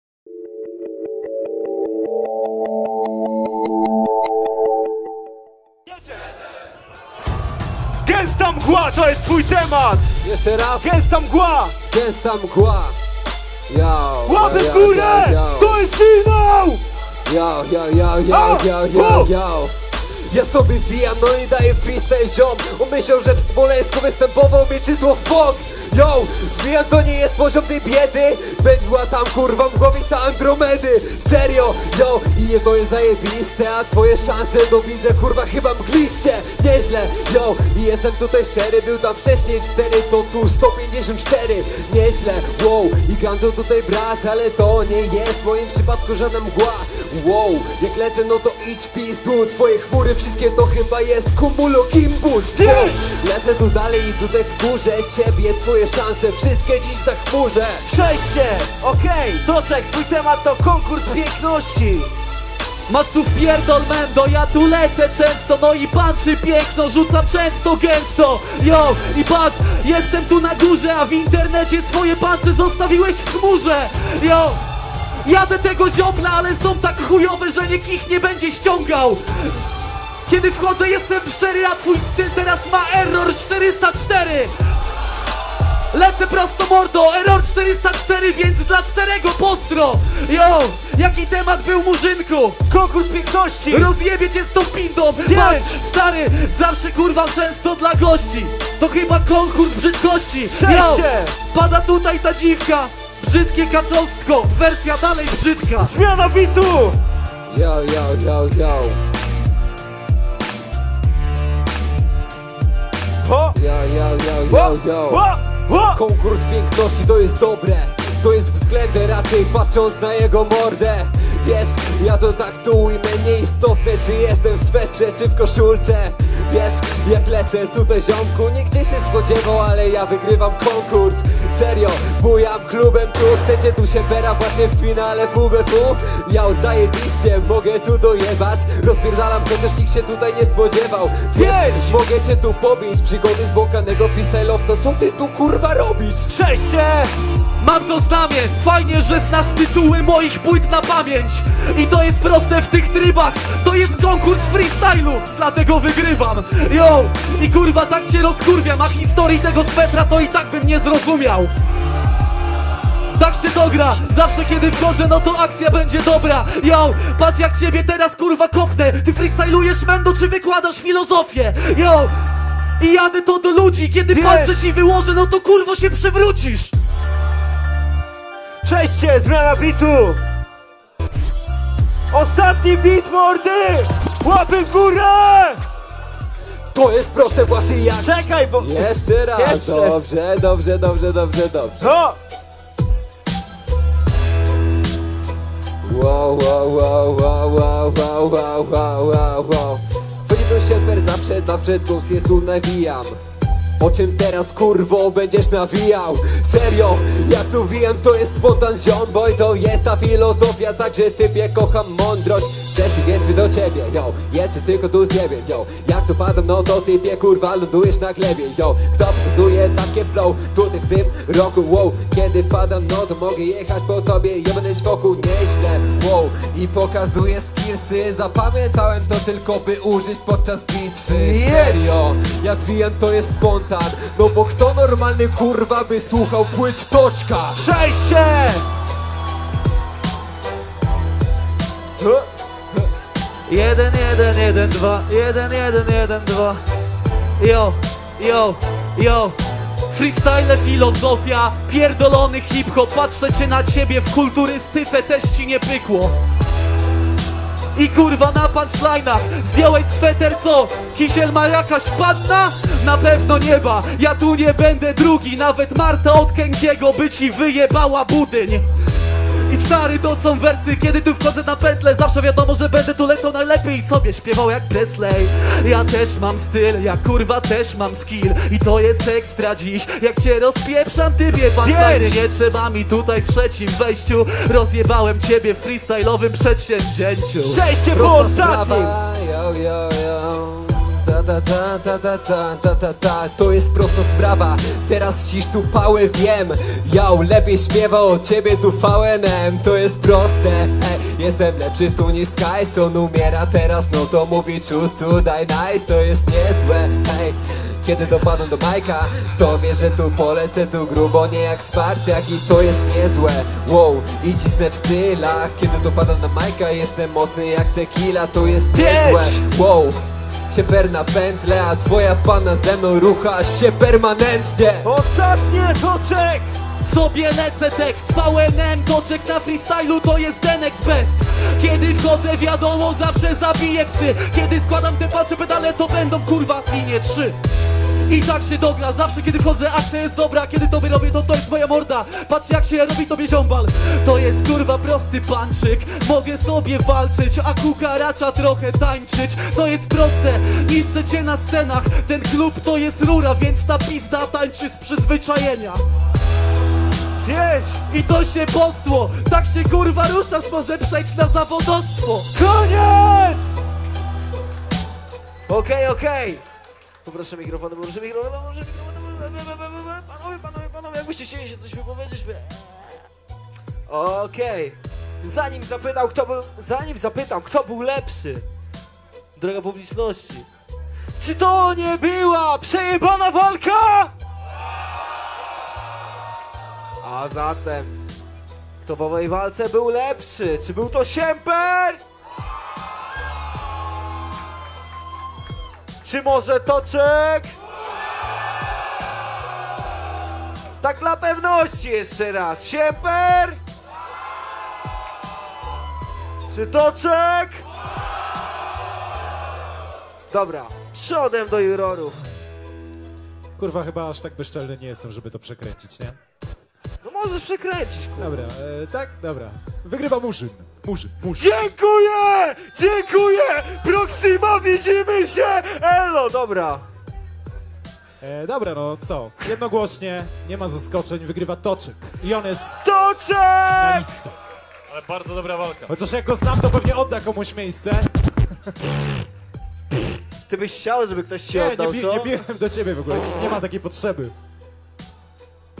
Toczek 🆚 Siemper 🎤 WBW 2019 Częstochowa (FINAŁ) freestyle rap battle
Wspieraj nasz kanał i oglądaj wszystkie walki od razu ☝️☝️☝️

freestyle rap battle: Toczek vs Siemper (finał)
prowadzenie: Murzyn
jury: Czeski, Mikser
gramofony: DJ Avens

🏆 WBW 2019 Częstochowa 🎤 Freestyle Battle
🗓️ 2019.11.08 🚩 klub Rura